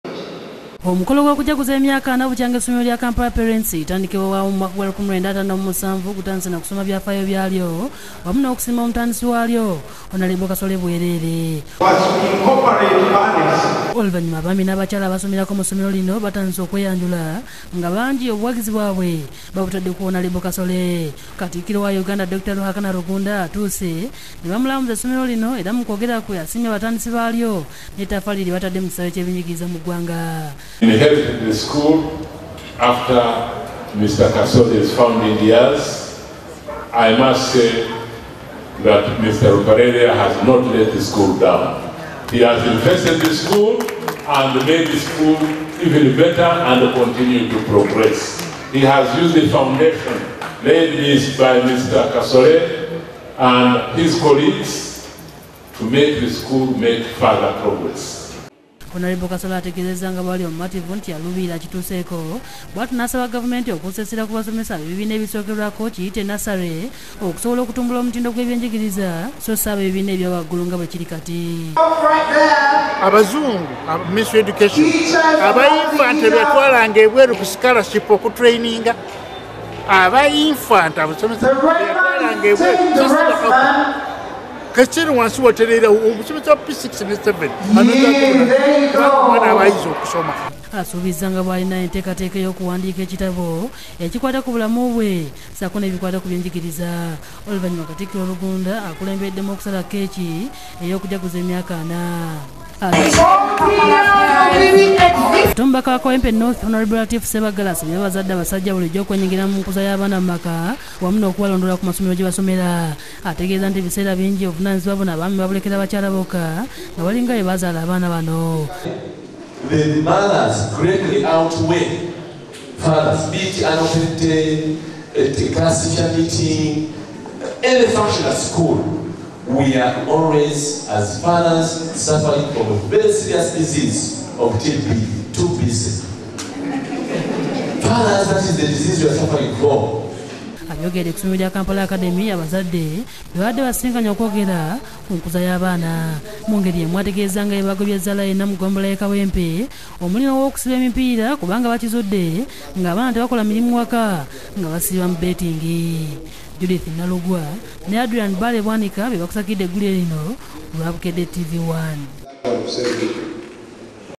Aba Kampala Parents bajaguzza emyaka 40
Basiimye emirimu gya Kasole Lwanga